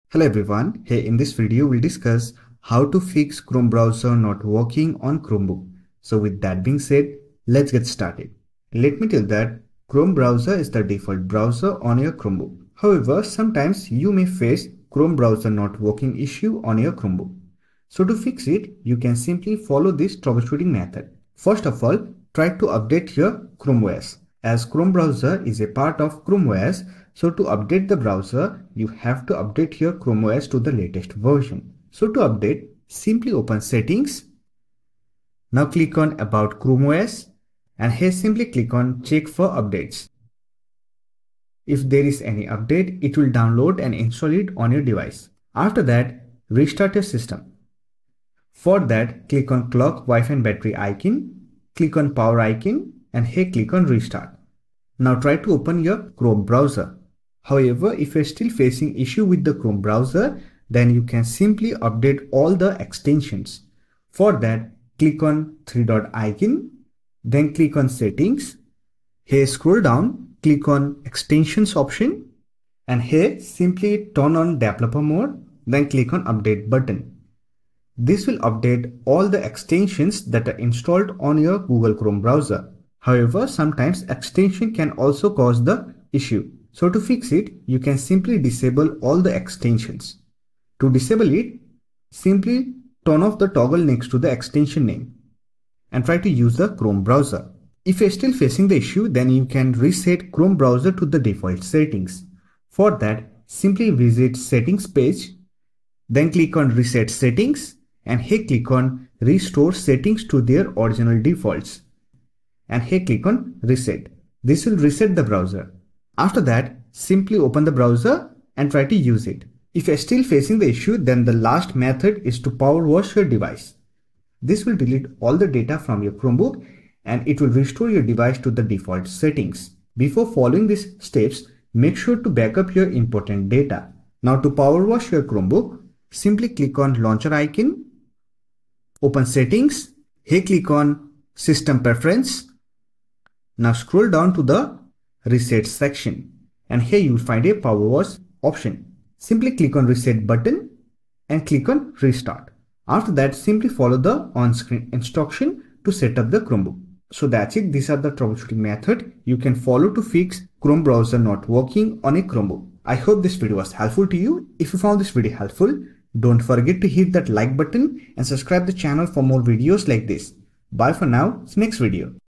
How to Fix Chrome Browser Not Working on Chromebook
In this video, we will learn how to fix the Chrome browser not working on a Chromebook. The Google Chrome browser is deeply integrated with Chrome OS and is also the default browser on Chromebooks. Sometimes, you might encounter issues with the Chrome browser. Here’s a quick video to help you fix it.